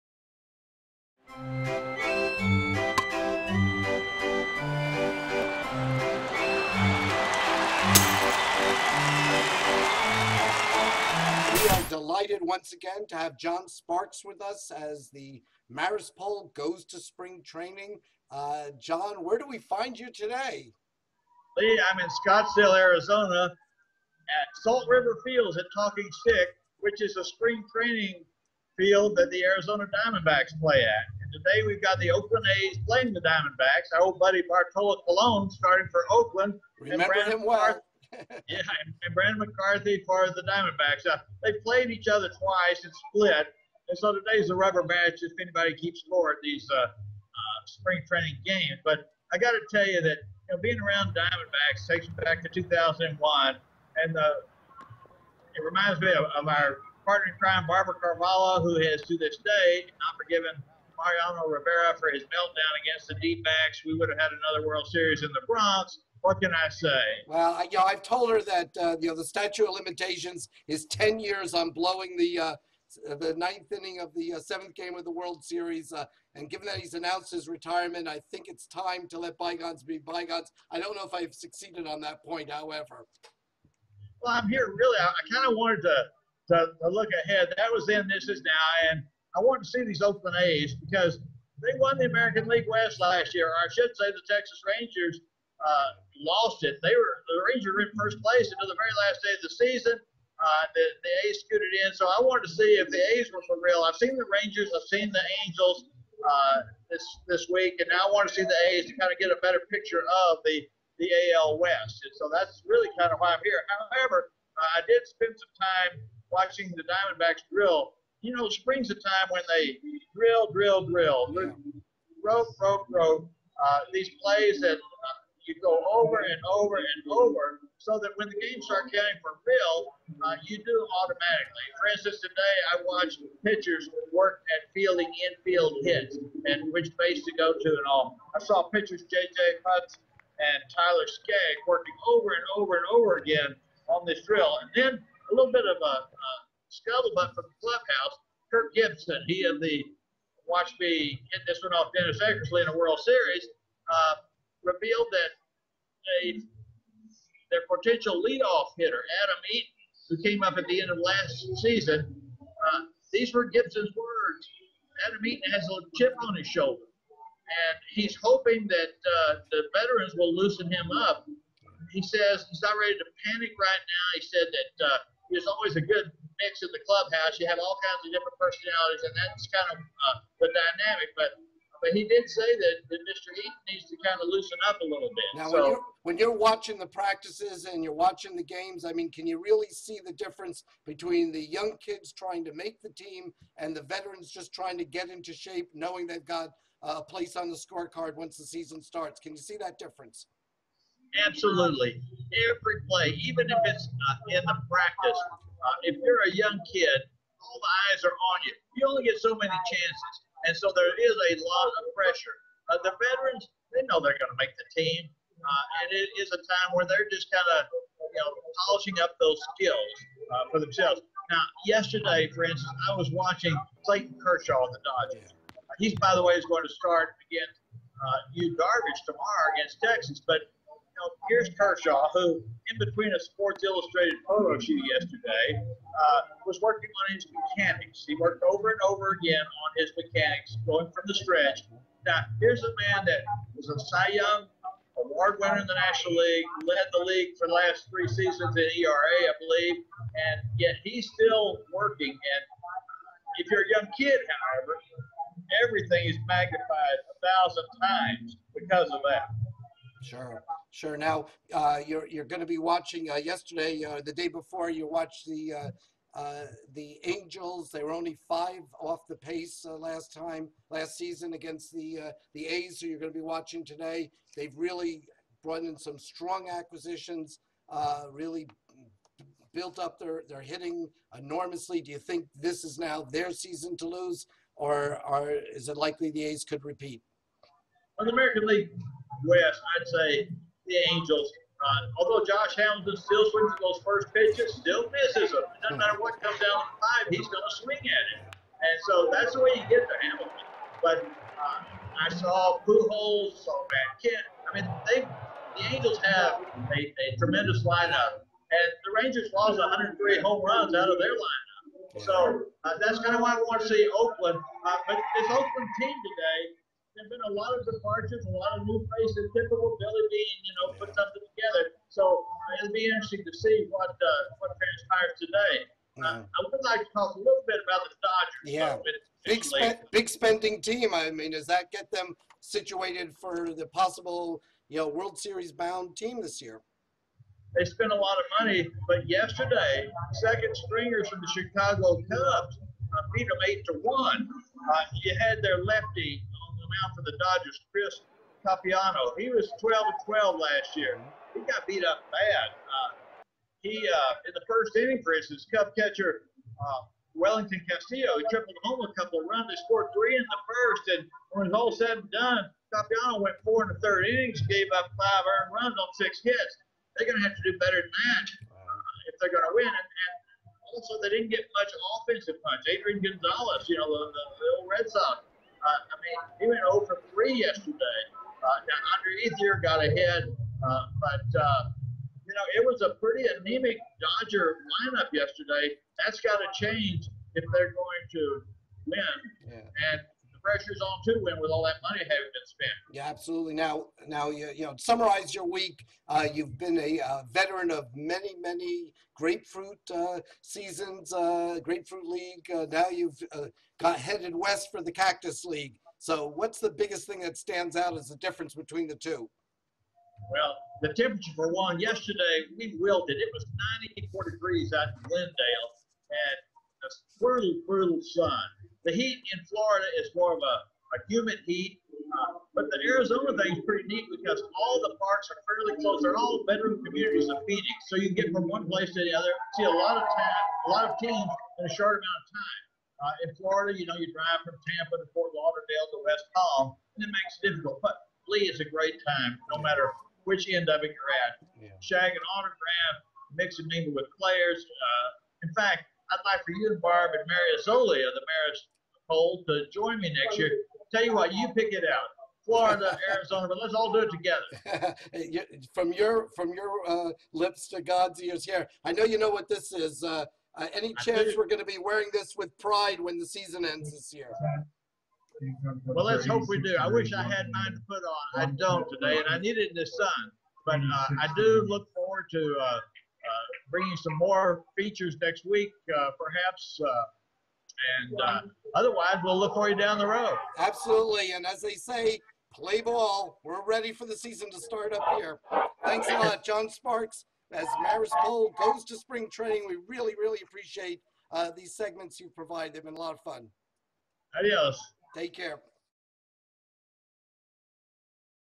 Baseball Spring Training
Lee Miringoff talks with John Sparks on baseball Spring training. March 15, 2013